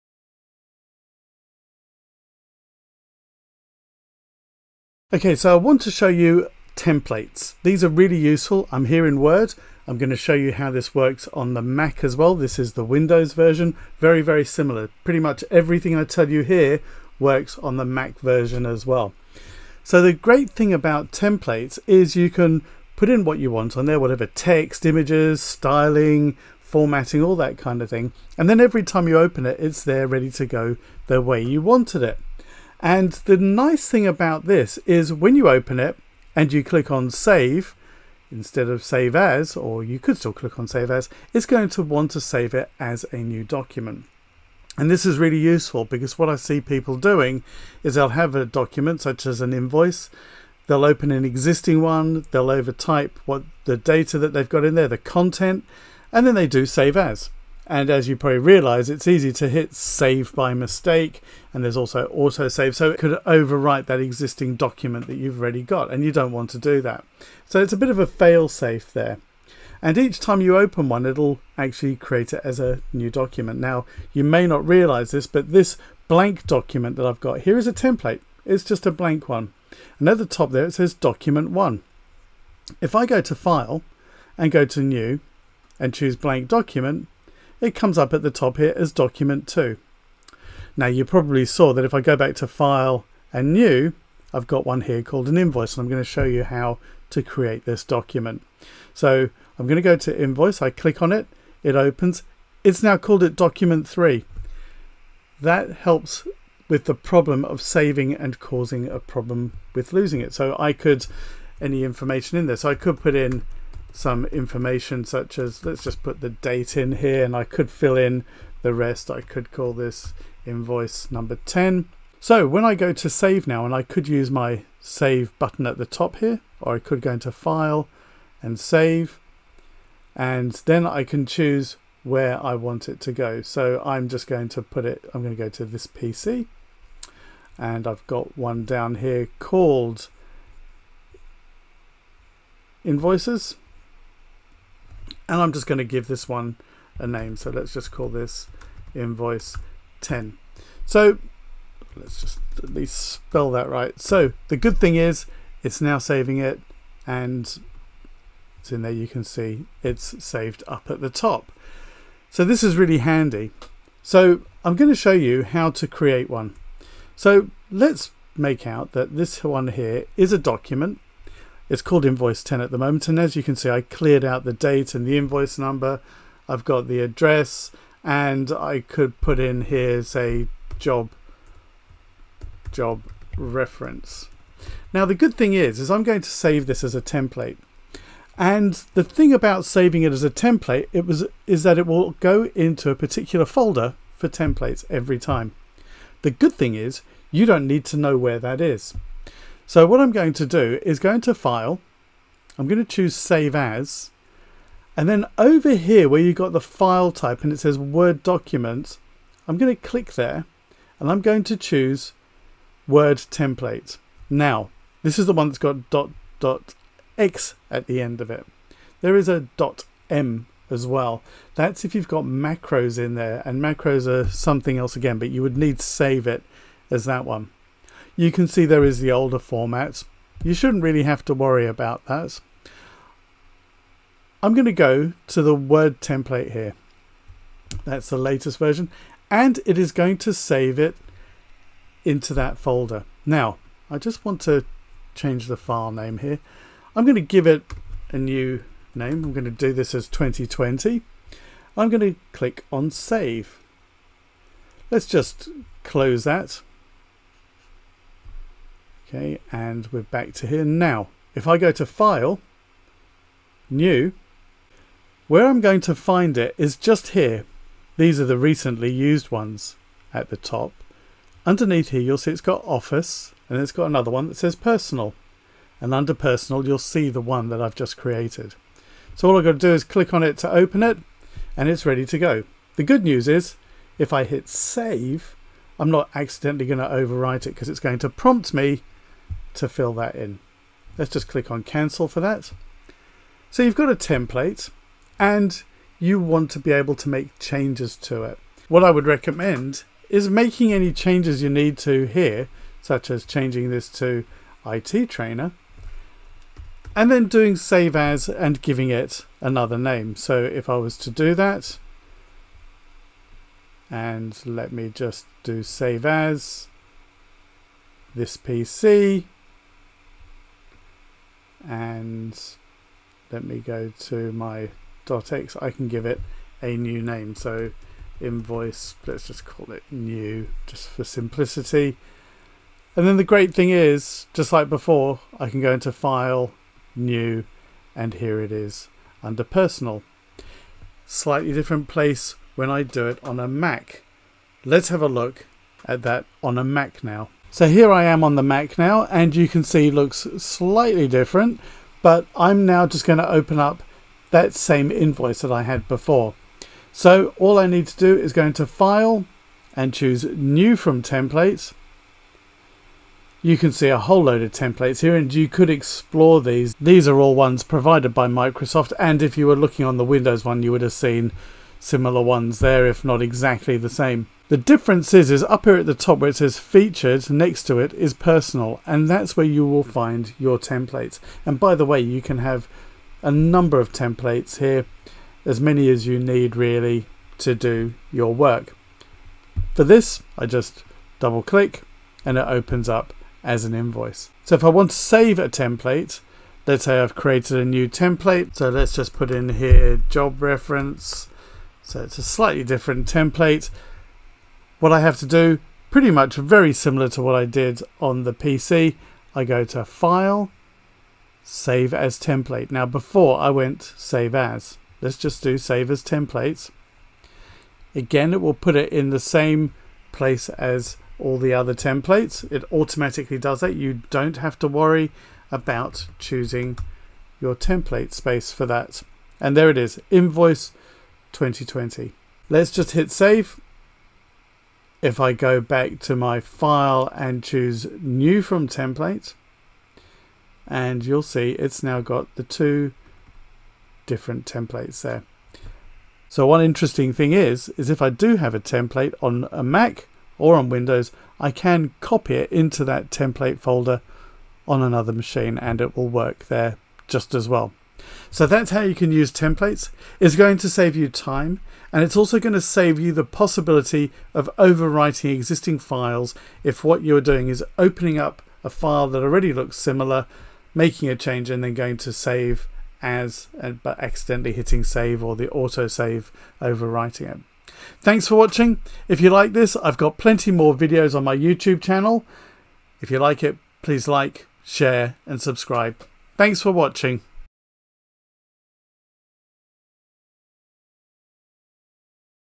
Creating Templates in Word
Microsoft Word templates are a great time saver and can speed up how you work. This is really useful for documents that you need to create often such as invoices, purchases orders, quotes or anything else that has content in a preset style you have made. The great thing about this is it helps if you usually open an existing document and run the risk of overwriting it if you click on Save rather than Save as.